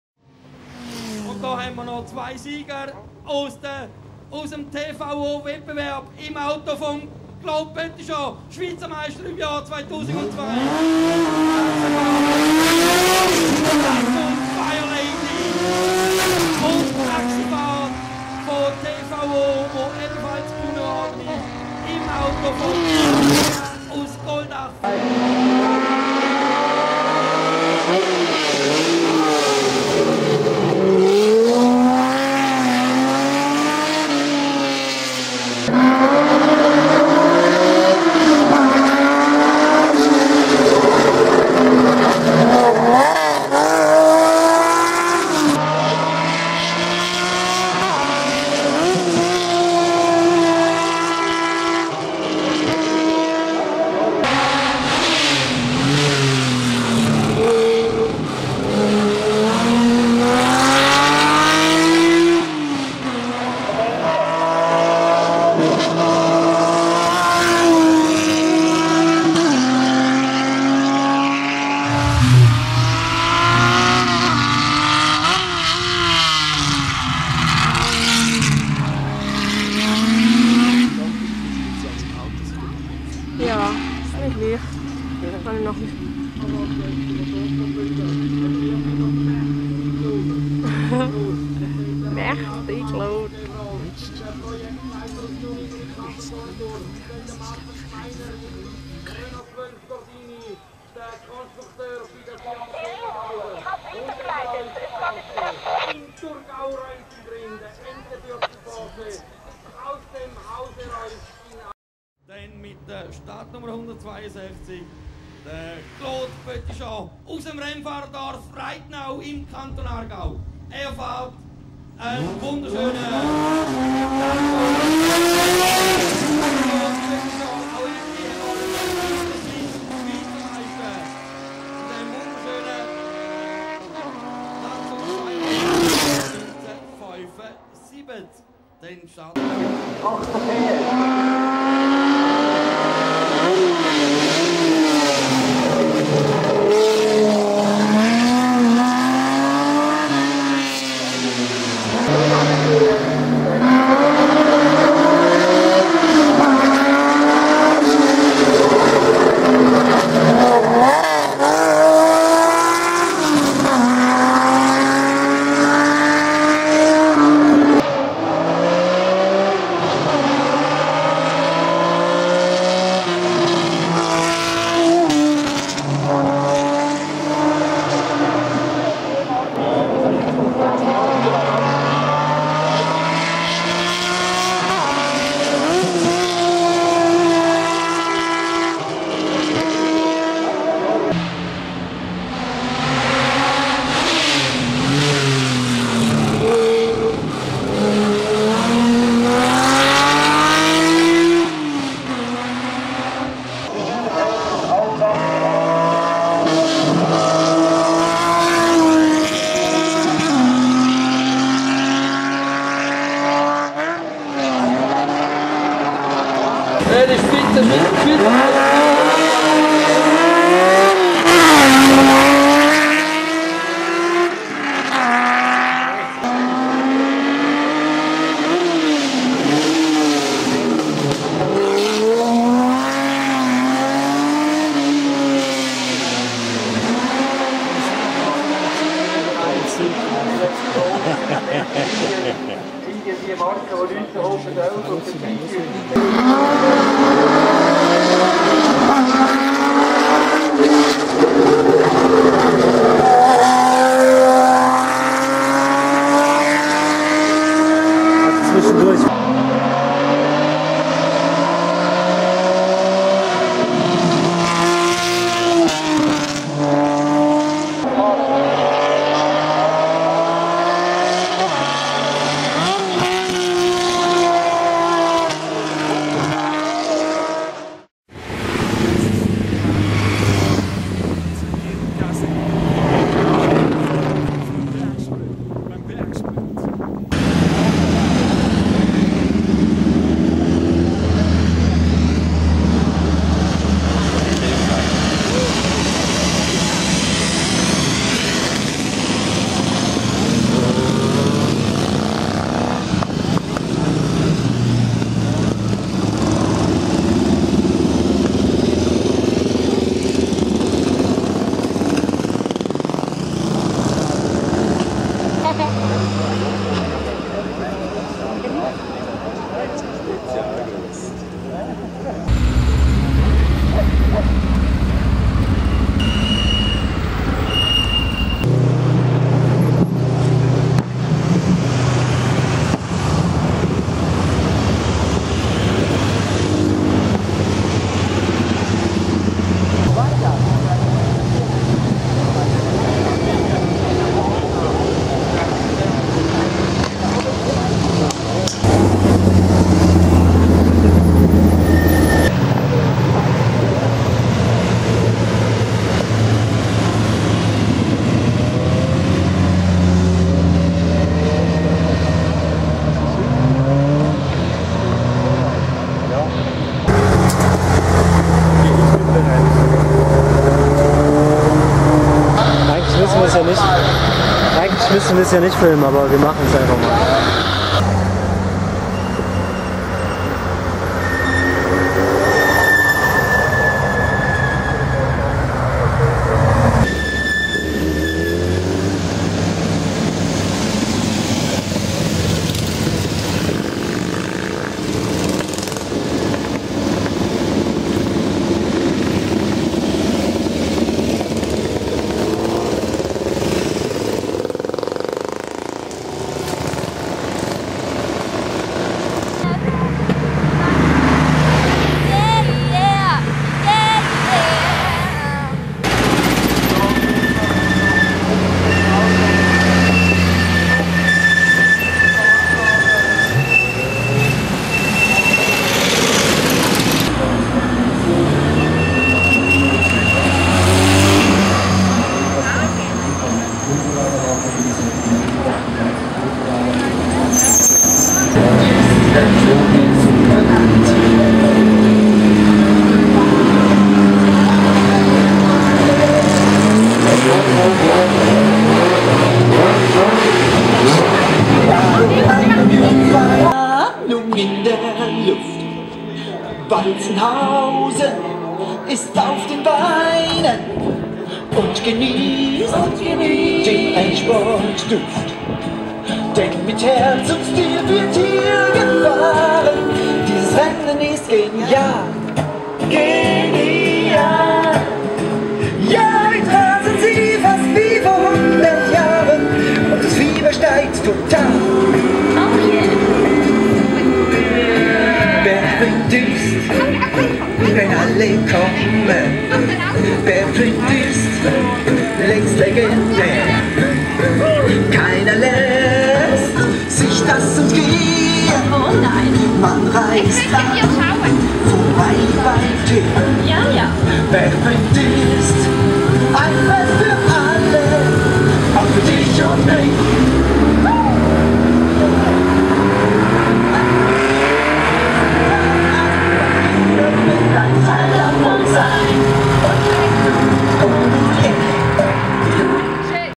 Z-Car Racing Team Walzenhausen 2013
Eine kleine wünderschöne Videoaufnahme vom Bergsprint Walzenhausen 2013
Welche wir vom Panorama Film Team geschenkt bekamen. Vielen Dank dafür.